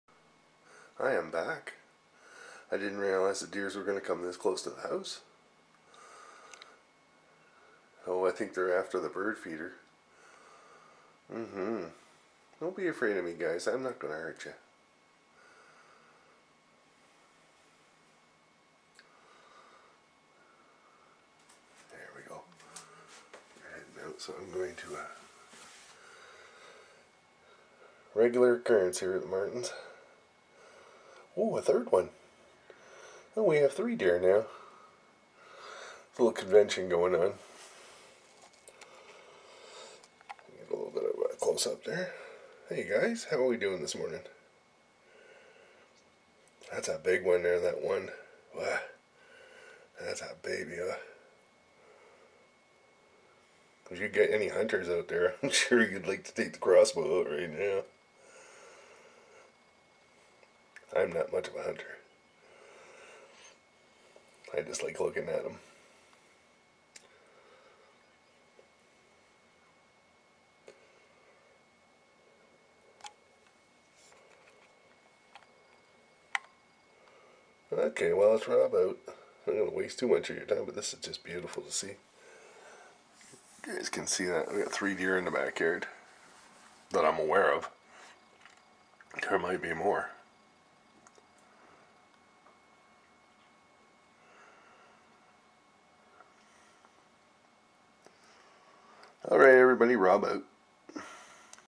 Deer Convention...
Gathering of white tailed deer in my backyard.  I was going to put the dog and cats out and there they were!